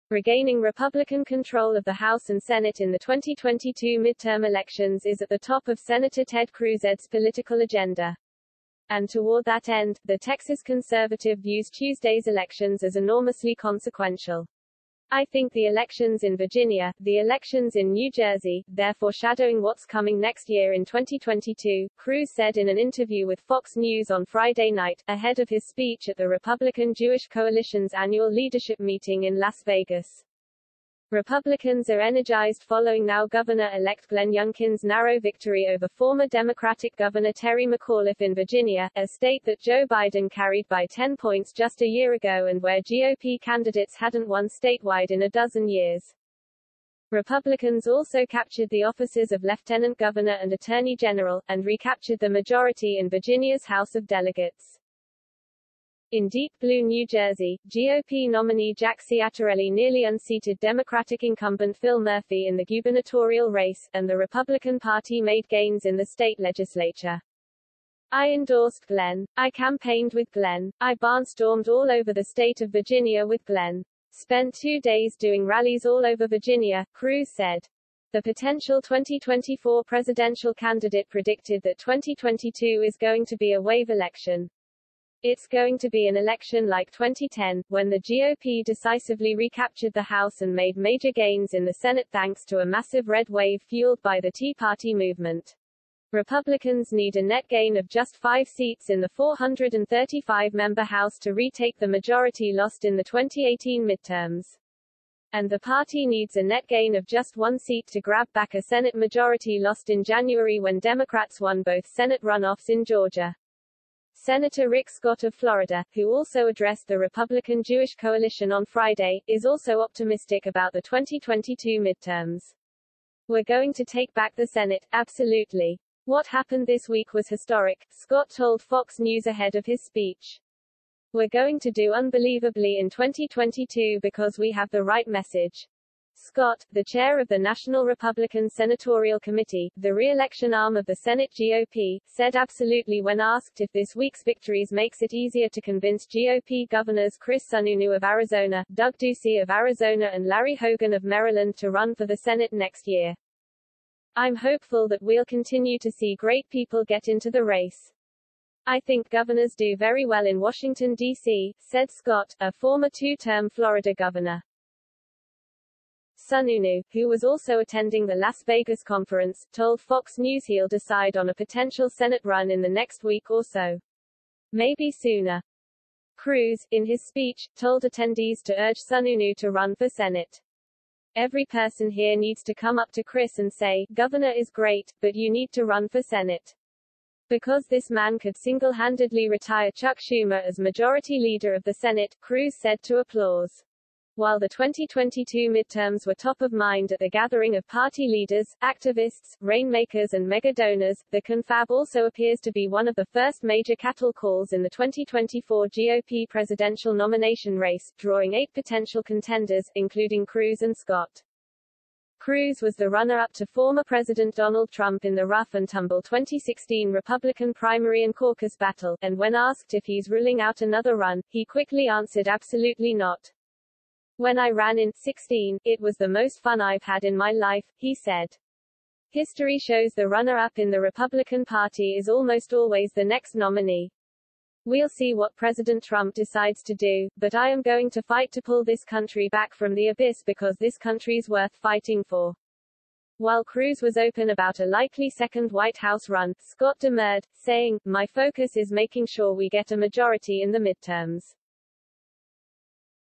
Cruz calls 2021 elections 'enormously consequential,' does not rule out 2024 run
Texas senator says elections in Virginia, New Jersey are 'foreshadowing what's coming next year in 2022'